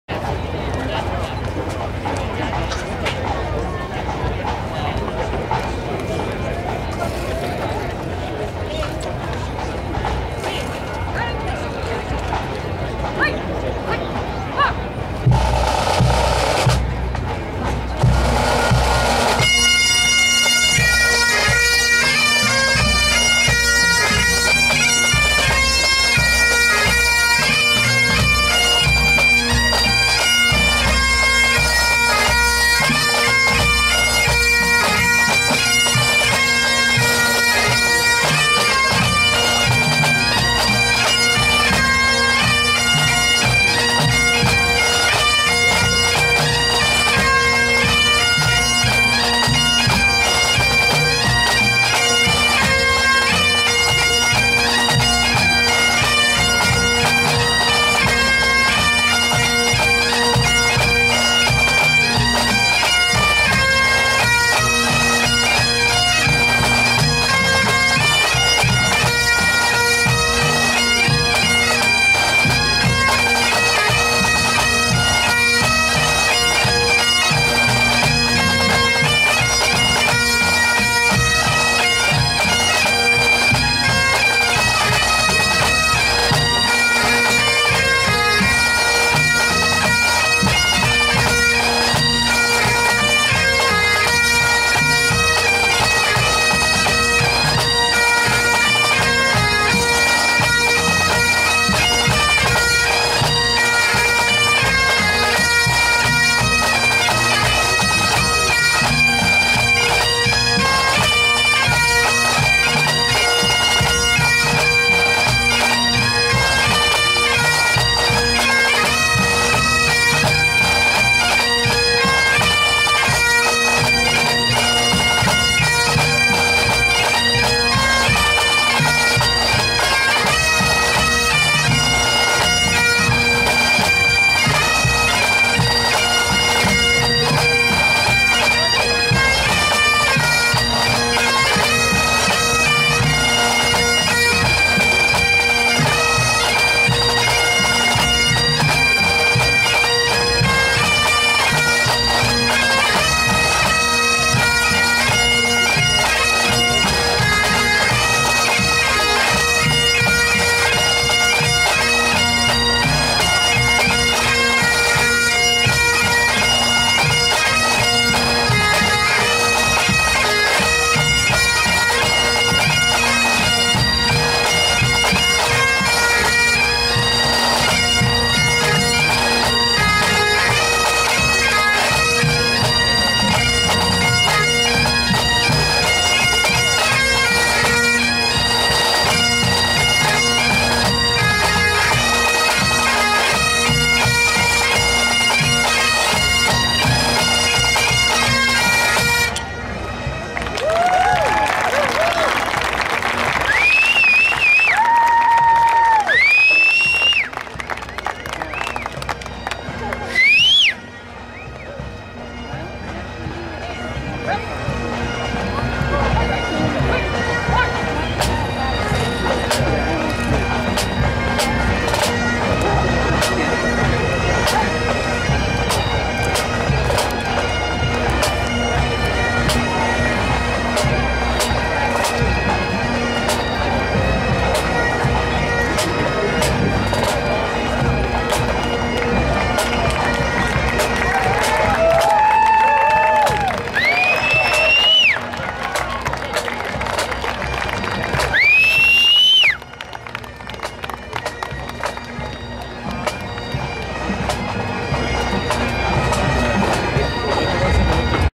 RI Highlanders New Hampshire Highland Games 2011 - QMM
The RI Highlanders played their QMM at Loon Mtn this year. Placed 3rd in Grade IV. -9/17/11